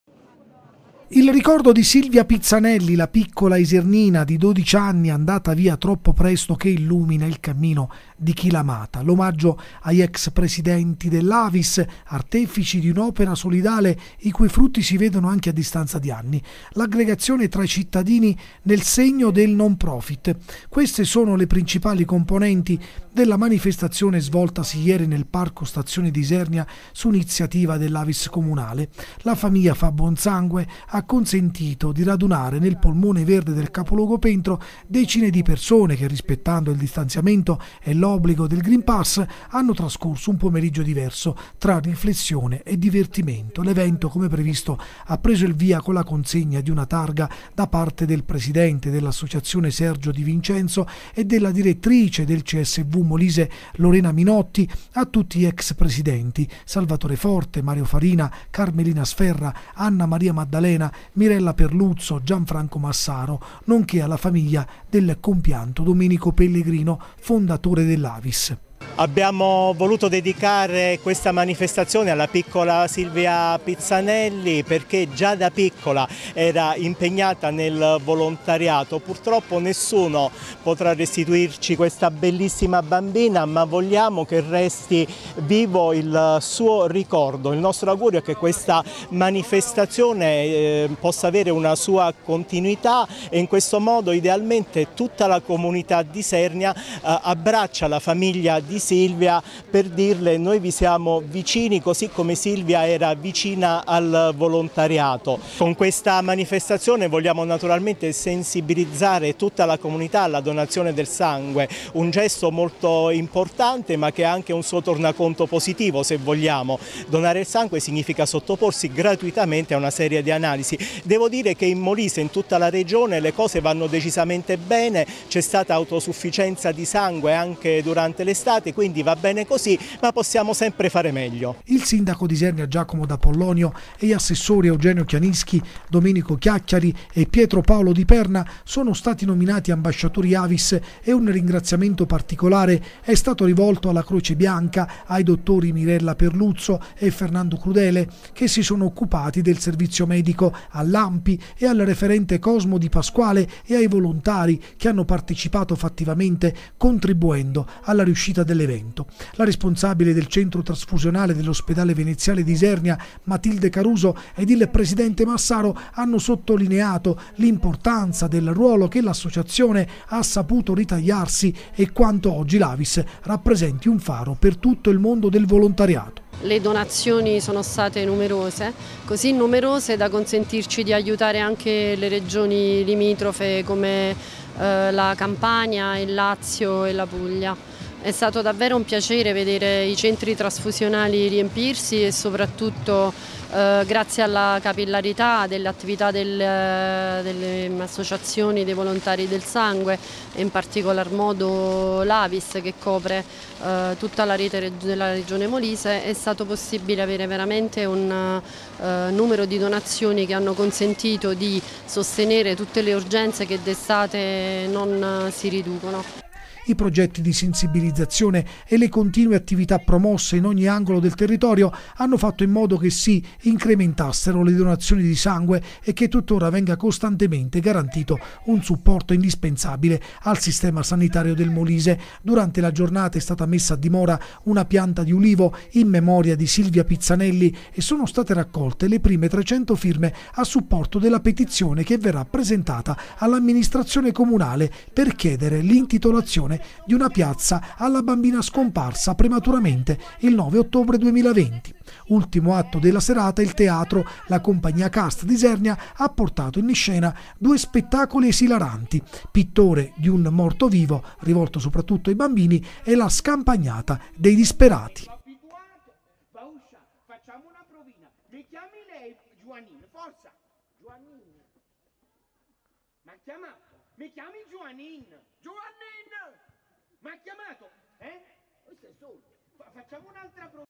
La famiglia fa buon sangue e crea solidarietà: ottimi riscontri per l'evento Av...
Servizi TG | La famiglia fa buon sangue e crea solidarietà: ottimi riscontri per l'evento Avis di Isernia | 6/9/2021